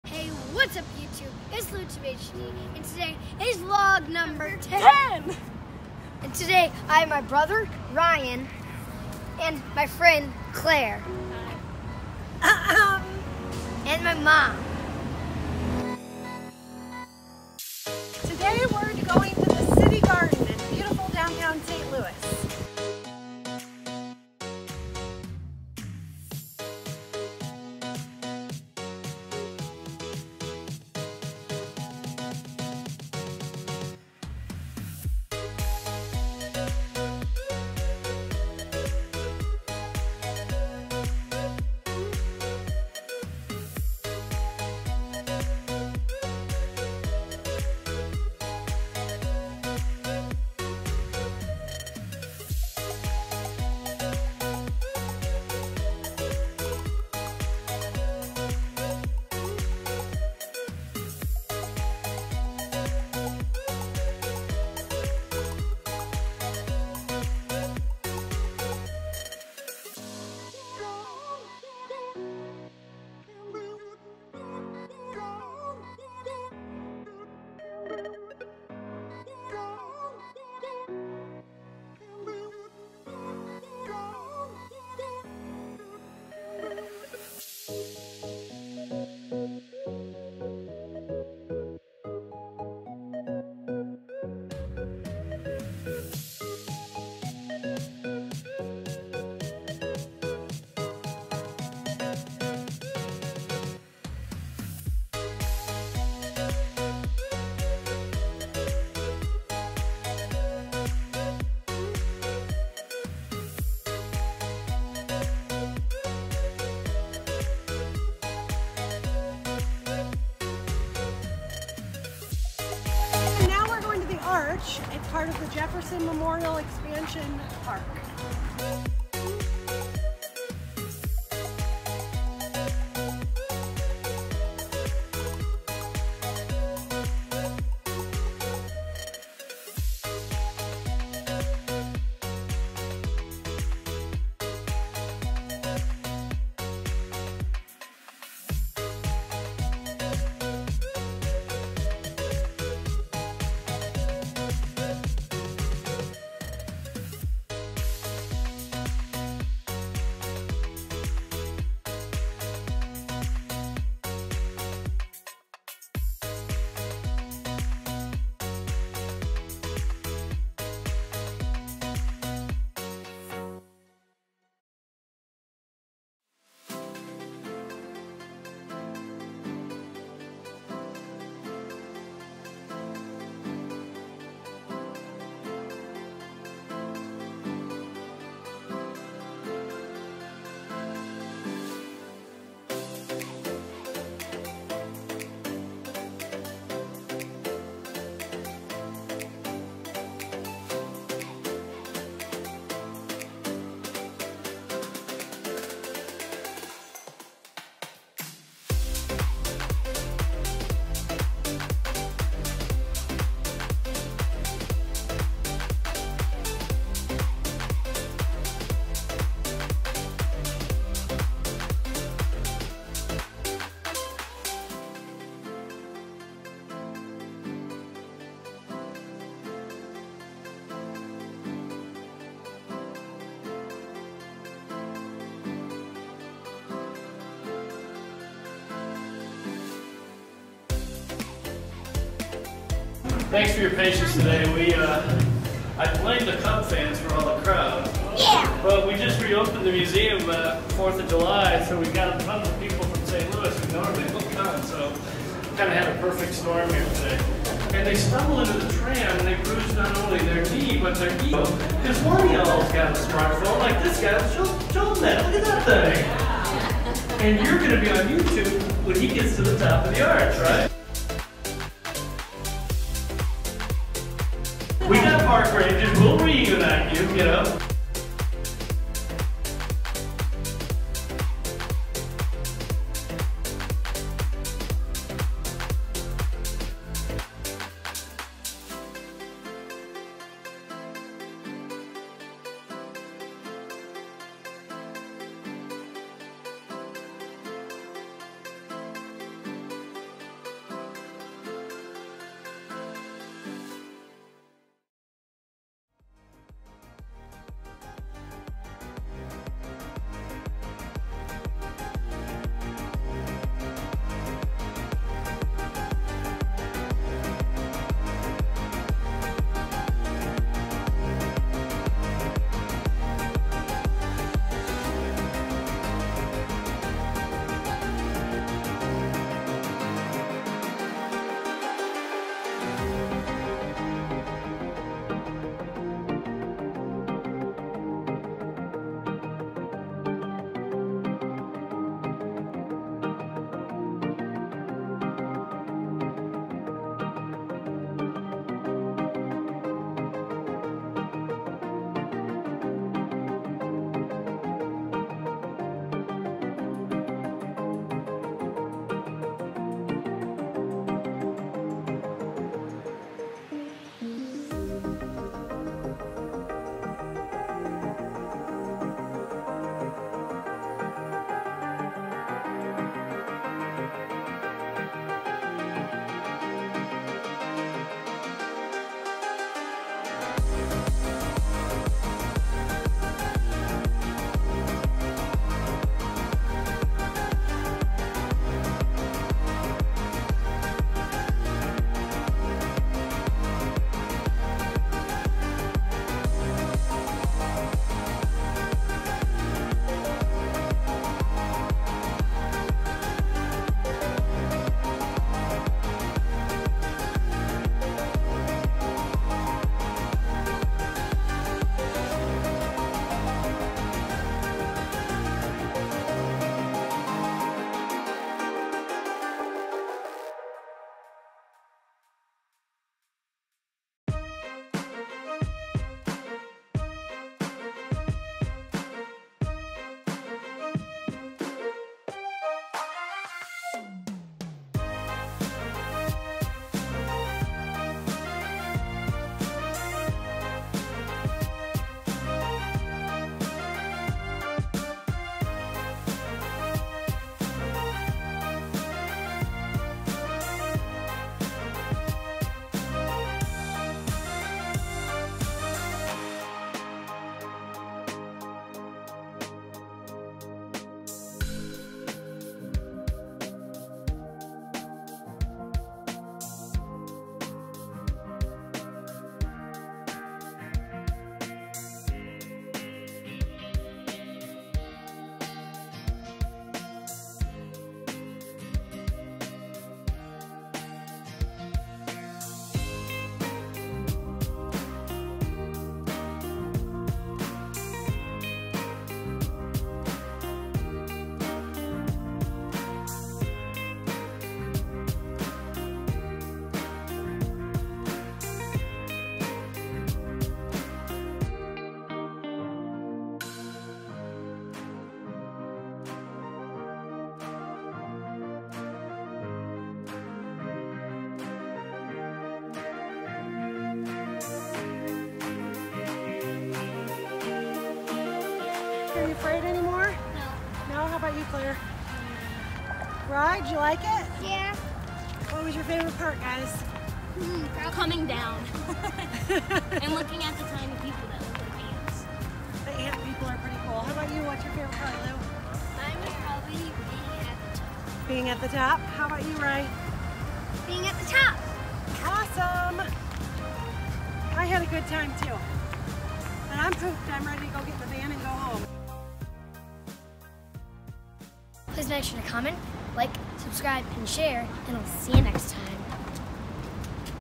Vlog #10: The Arch Adventure!
In this video my Mom, Clair, Ryan and I go up in the St. Louis Arch for the very first time!